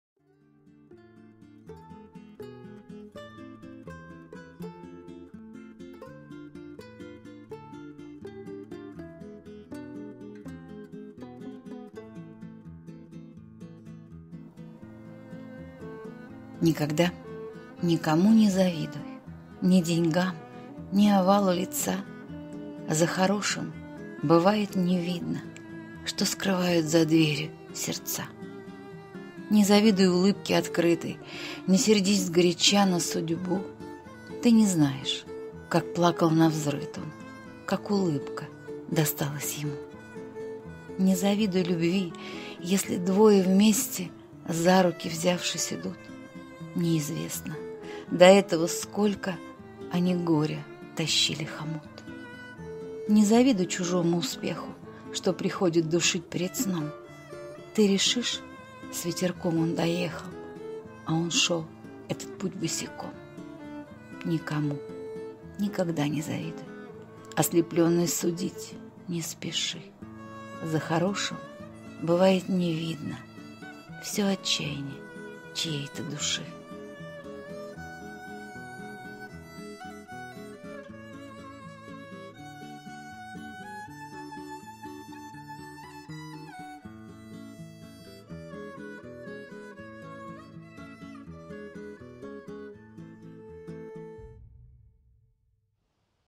Никогда никому не завидуй (Инесса Федотовская)
Никогда никому не завидуй,
Ни деньгам, ни овалу лица,
За хорошим бывает не видно,
Что скрывают за дверью сердца.

Не завидуй улыбке открытой,
Не сердись сгоряча на судьбу,
Ты не знаешь, как плакал навзрыд он,
Как улыбка досталась ему.

Не завидуй любви, если двое
Вместе за руки взявшись идут,
Неизвестно до этого сколько,
Они горя тащили хомут.

Не завидуй чужому успеху,
Что приходит душить перед сном,
Ты решишь — с ветерком он доехал,
А он шёл этот путь босиком…

Никому никогда не завидуй,
Ослепленный судить не спеши…
Всё отчаянье чьей-то души.